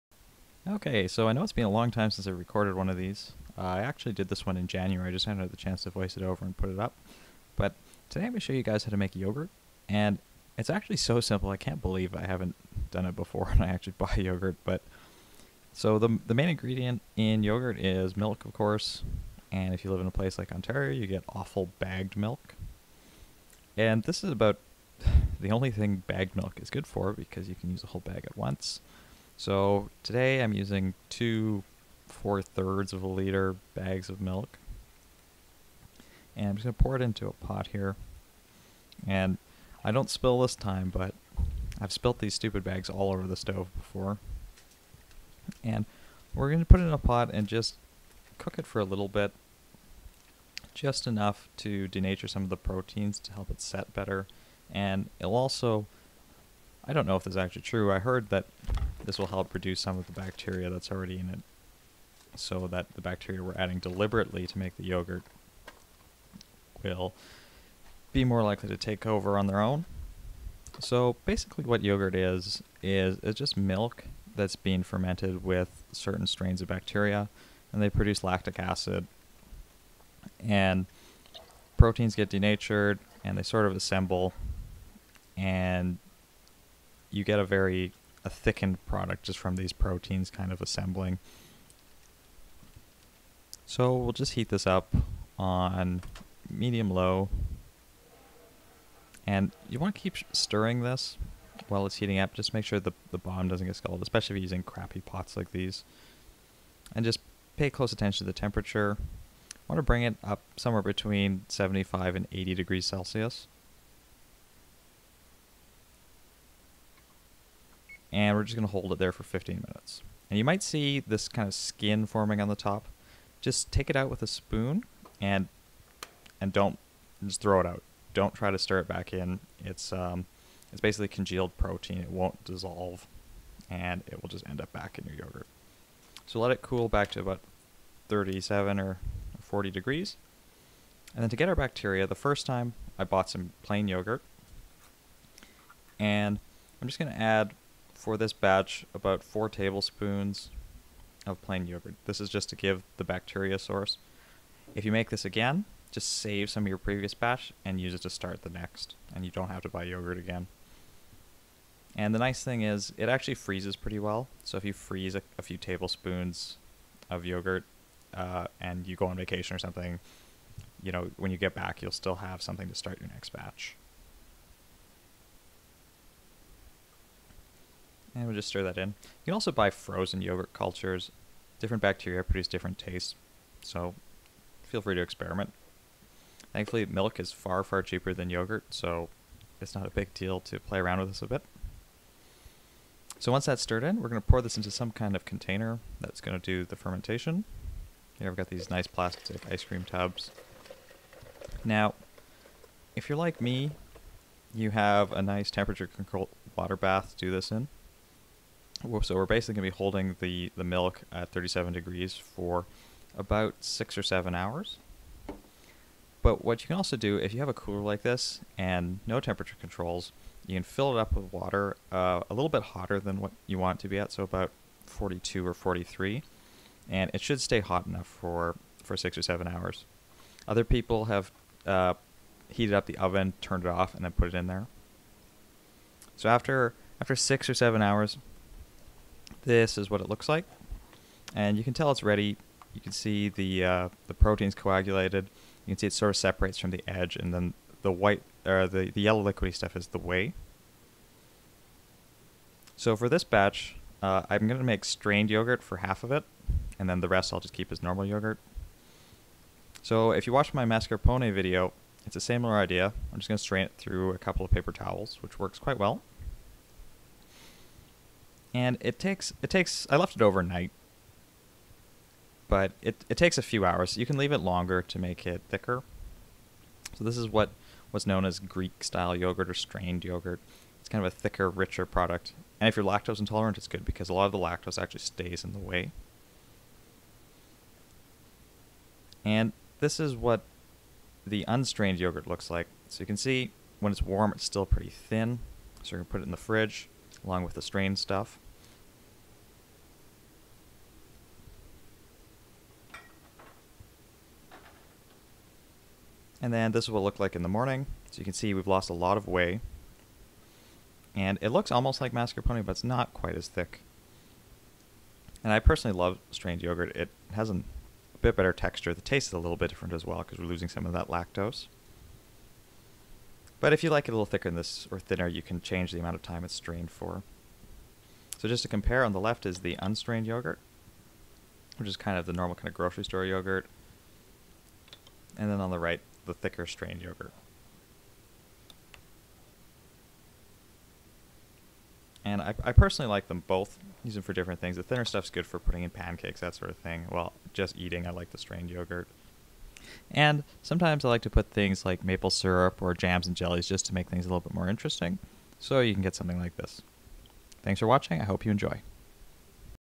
How to Make Yogurt (and return of the dino PJs)
I really love yogurt, but it's stupidly expensive for what it is and it's so easy to make yourself. Yogurt is essentially milk fermented by lactic acid producing bacteria. The acid coagulates milk proteins, causing thickening (similar to how mascarpone cheese is made). All we have to do here is bring the milk up to around 80 C to denature some of the whey proteins (which helps thicken the yogurt, especially if part skim milk is used) and kill some of the bacteria already present, then allow it to cool to our culture temperature, 37 C, and add a source of bacteria. Storebought yogurt or small samples of the last batch of homemade yogurt work well, but many different varieties of frozen yogurt starters are available with different mixes of bacteria producing different tastes.

The mixture is then held at 37 C for 6-7 hours to ferment. Longer times produce thicker yogurt, but the bacteria produce more acid making a more sour taste. I use my sous vide setup to ferment, but if you have just an insulated cooler, filling it with ~42 C water will keep it warm enough for a few hours. Some people also heat their ovens up a bit and do it in there.

The end product, once cooled, is similar in consistency to commercially-available yogurt, but can also be strained through a cheesecloth or paper towel to make Greek-style yogurt, which is much thicker and contains less lactose (which mostly remains in the liquid whey that is separated out).

Higher fat milk makes thicker and richer yogurt, but skim milk will work too. I generally like 2% (mostly because it's cheaper than homogenized).

If you want to add some flavour, try adding maple syrup, honey, jam, jelly, small fruits, etc.

You'll need:
2.7 L milk
60 mL plain storebought or homemade yogurt (or yogurt starter)

Don't forget to set some of your batch aside for the next one!